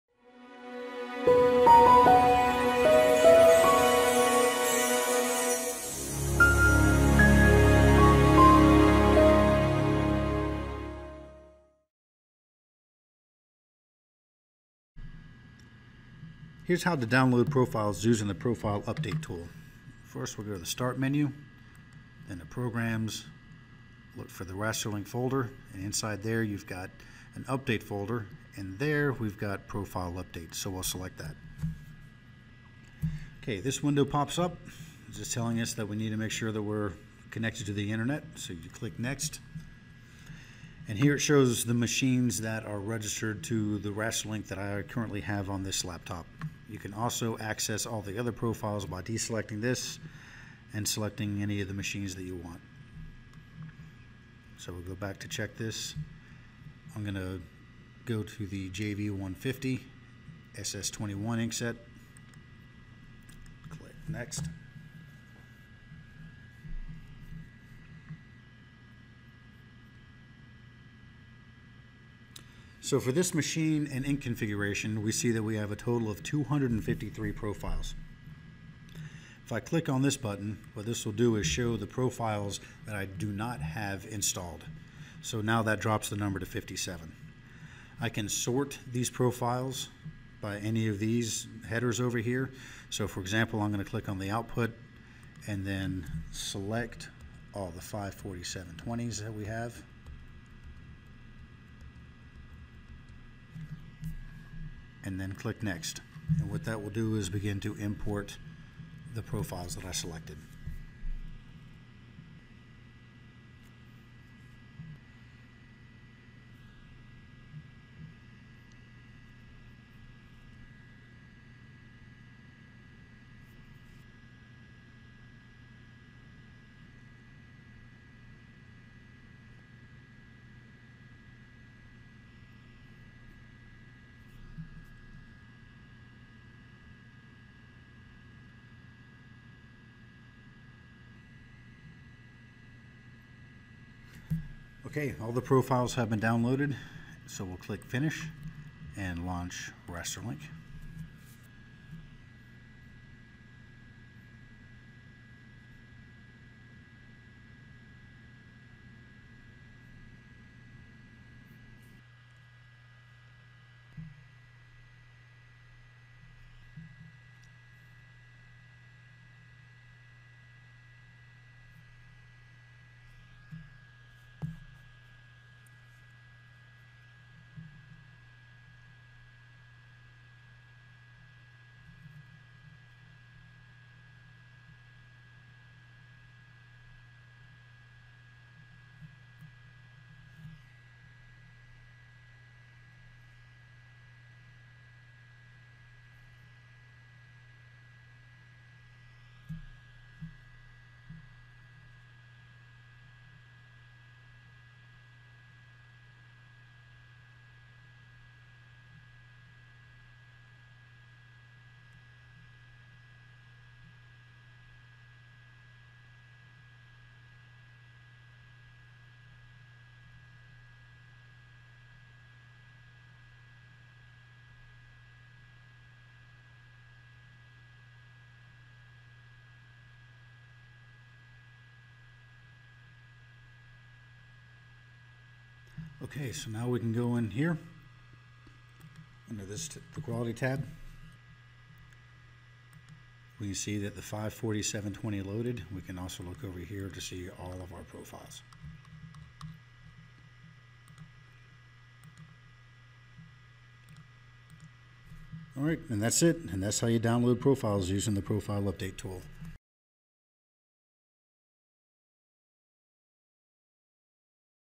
RasterLink6 Profile Update Tool
Marc from Mimaki USA walks through the very simple process of adding new or updated color profiles to your Mimaki RasterLink6 RIP using the Profile Update tool.